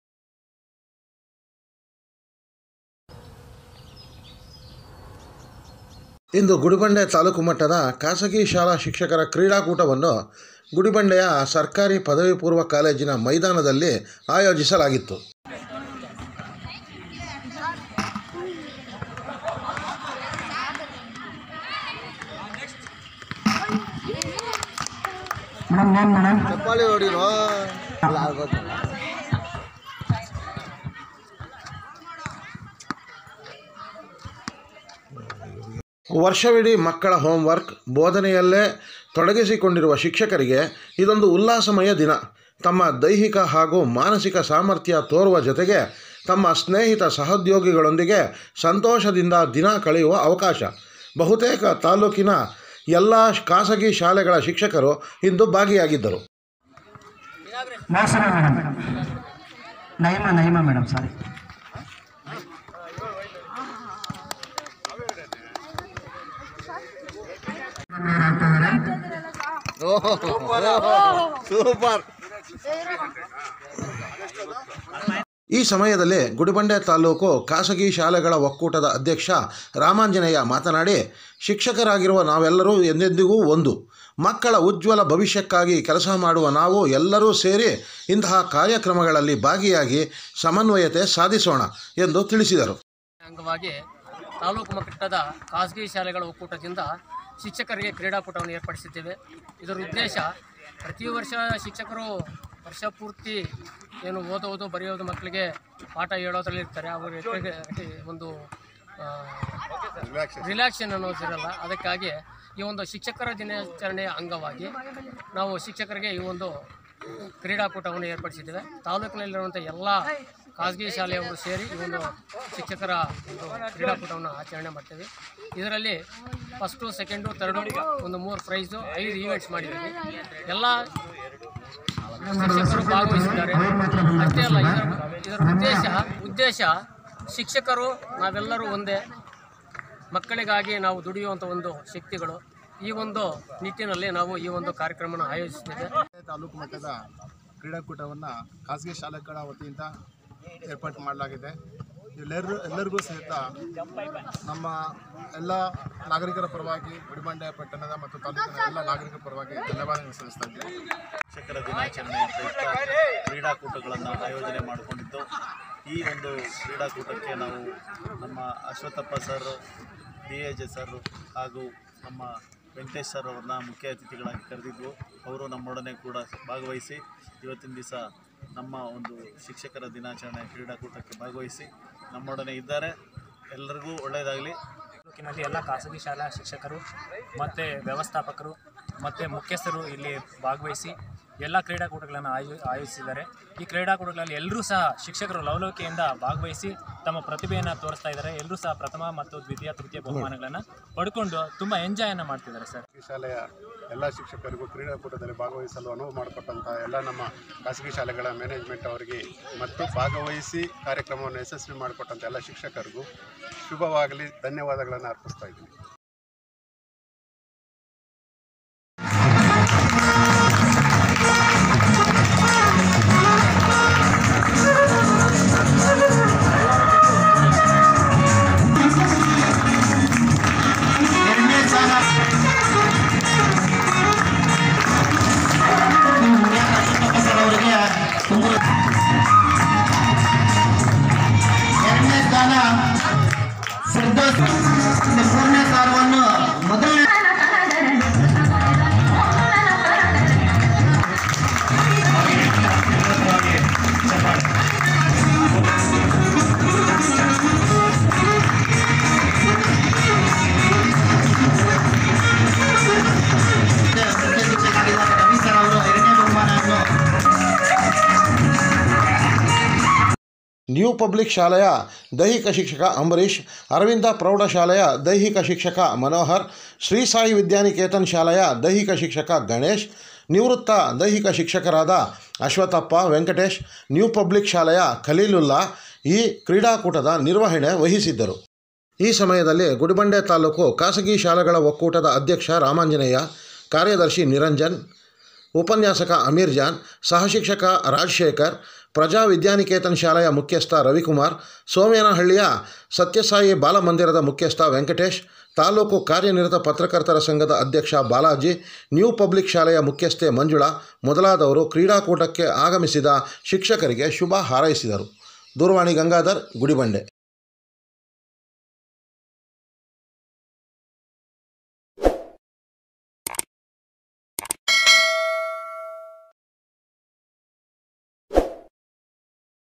ಗುಡಿಬಂಡೆ ಸರ್ಕಾರಿ ಪದವಿಪೂರ್ವ ಕಾಲೇಜಿನ ಮೈದಾನದಲ್ಲಿ ನಡೆದ ತಾಲ್ಲೂಕು ಖಾಸಗಿ ಶಾಲಾ ಶಿಕ್ಷಕರ ಕ್ರೀಡಾಕೂಟ.
ಇಂದು ಗುಡಿಬಂಡೆ ತಾಲ್ಲೂಕು ಮಟ್ಟದ ಖಾಸಗಿ ಶಾಲಾ ಶಿಕ್ಷಕರ ಕ್ರೀಡಾಕೂಟವನ್ನು ಗುಡಿಬಂಡೆಯ ಸರ್ಕಾರಿ ಪದವಿಪೂರ್ವ ಕಾಲೇಜಿನ ಮೈದಾನದಲ್ಲಿ ಆಯೋಜಿಸಲಾಗಿತ್ತು.

ವರ್ಷವಿಡೀ ಮಕ್ಕಳ ಹೋಂವರ್ಕ್, ಭೋದನೆ ಯಲ್ಲೇ ತೊಡಗಿಸಿಕೊಂಡಿರುವ ಶಿಕ್ಷಕರಿಗೆ ಇದೊಂದು ಉಲ್ಲಾಸಮಯ ದಿನ. ತಮ್ಮ ದೈಹಿಕ ಹಾಗೂ ಮಾನಸಿಕ ಸಾಮರ್ಥ್ಯ ತೊರುವ ಜೊತೆಗೆ ತಮ್ಮ ಸ್ನೇಹಿತ ಸಹೋದ್ಯೋಗಿಗಳೊಂದಿಗೆ ಸಂತೋಷದಿಂದ ದಿನ ಕಳೆಯುವ ಅವಕಾಶ. ತಾಲ್ಲೂಕಿನ ಏಳು ಖಾಸಗಿ ಶಾಲೆಗಳ ಶಿಕ್ಷಕರು ಇಂದು ಭಾಗಿಯಾಗಿದ್ದರು.

ಅರವಿಂದ ಫ್ರೌಡಶಾಲೆಯ ದೈಹಿಕ ಶಿಕ್ಷಕ  ಮನೋಹರ್, ನ್ಯೂ ಪಬ್ಲಿಕ್ ಶಾಲೆಯ ದೈಹಿಕ ಶಿಕ್ಷಕ ಅಂಬರೀಶ್, ಶ್ರೀ ಸಾಯಿ ವಿದ್ಯಾನಿಕೇತನ್ ಶಾಲೆಯ. ದೈಹಿಕ ಶಿಕ್ಷಕ ಗಣೇಶ್,  ನಿವೃತ್ತ ದೈಹಿಕಶಿಕ್ಷಕರಾದ ಅಶ್ವತ್ಥಪ್ಪ, ವೆಂಕಟೇಶ್, ನ್ಯೂ ಪಬ್ಲಿಕ್ ಶಾಲೆಯ ಖಲೀಲುಲ್ಲಾ ಈ ಕ್ರೀಡಾಕೂಟದ ನಿರ್ವಹಣೆ ವಹಿಸಿದ್ದರು.

ಈ ಸಮಯದಲ್ಲಿ ಗುಡಿಬಂಡೆ ತಾಲ್ಲೂಕು ಖಾಸಗಿ ಶಾಲೆಗಳ ಒಕ್ಕೂಟದ ಅಧ್ಯಕ್ಷ ರಾಮಾಂಜನೇಯ,     ಕಾರ್ಯದರ್ಶಿ ನಿರಂಜನ್, ಕ್ರೀಡಾಕೂಟದ  ಉಪನ್ಯಾಸಕ ಅಮೀರ್ ಜಾನ್, ಸಹಶಿಕ್ಷಕ ರಾಜಶೇಖರ, ಪ್ರಜಾ ವಿದ್ಯಾನಿಕೇತನ್ ಶಾಲೆಯ ಮುಖ್ಯಸ್ಥ ರವಿಕುಮಾರ್, ಸೋಮೇನಹಳ್ಳಿ ಸತ್ಯಸಾಯಿ ಬಾಲಮಂದಿರದ ಮುಖ್ಯಸ್ಥ ವೆಂಕಟೇಶ್, ತಾಲ್ಲೂಕು ಕಾರ್ಯನಿರತ ಪತ್ರಕರ್ತರ ಸಂಘದ ಅಧ್ಯಕ್ಷ ಬಾಲಾಜಿ, ನ್ಯೂ ಪಬ್ಲಿಕ್ ಶಾಲೆಯ ಮುಖ್ಯಸ್ಥೆ ಮಂಜುಳ ಮೊದಲಾದವರು ಕ್ರೀಡಾಕೂಟಕ್ಕೆ ಆಗಮಿಸಿದ ಶಿಕ್ಷಕರಿಗೆ ಶುಭ ಹಾರೈಸಿದರು.

ದೂರವಾಣಿ ಗಂಗಾಧರ್. ಗುಡಿಬಂಡೆ.